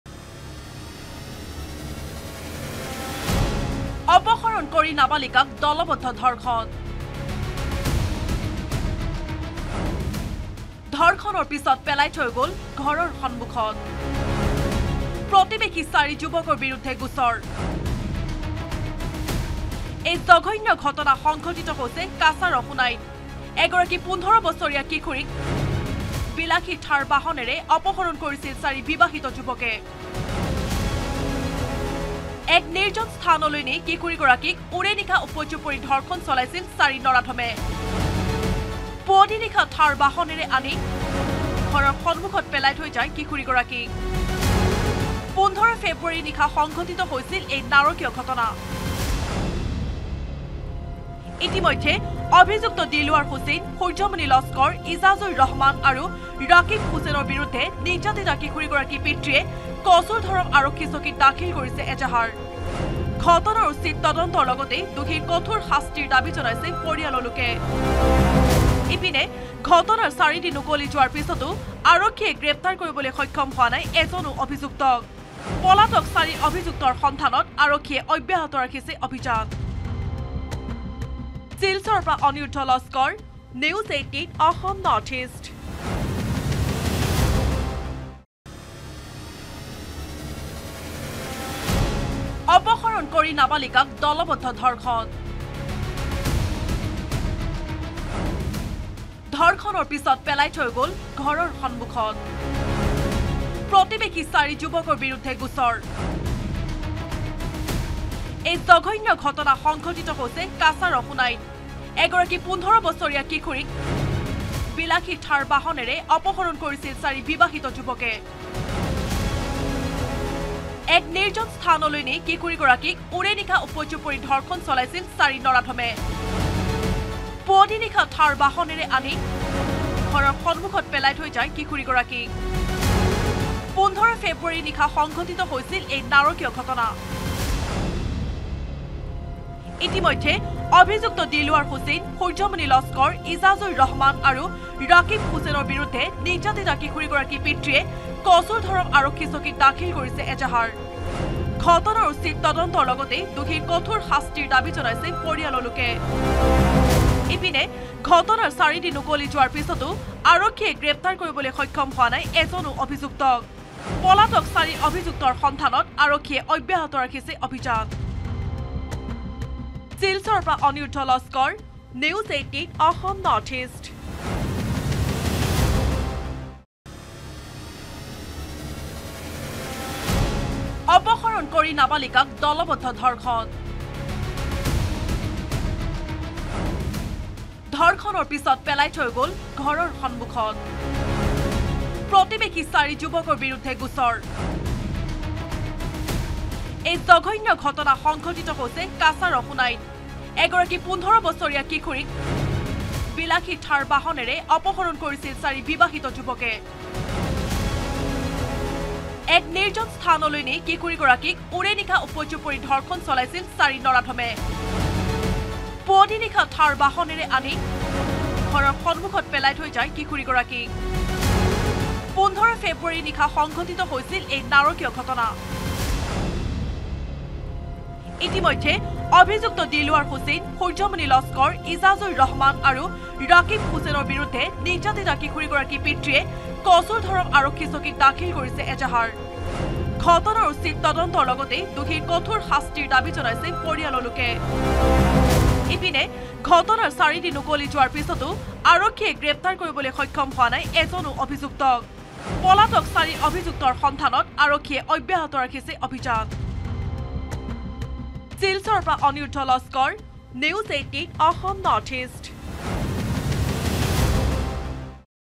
Cachar News | কাছাৰত সোণাইত সংঘটিত হৈছে এক জঘন্য ঘটনা | N18V
কাছাৰত সোণাইত সংঘটিত হৈছে এক জঘন্য ঘটনা। এগৰাকী কিশোৰীক অপহৰণ কৰি দলবদ্ধ ধৰ্ষণ চাৰি লম্পট বিবাহিত যুৱকৰ। ওৰেনিশা উপৰ্যুপৰি ধৰ্ষণ চলোৱাৰ পিছত ঘৰৰ সন্মুখত পেলাই থৈ যায় কিশোৰীগৰাকীক। ঘটনাৰ চাৰিদিন পিছতো এজনো অপৰাধীক গ্ৰেপ্তাৰ কৰিবলৈ সক্ষম নহ’ল আৰক্ষী।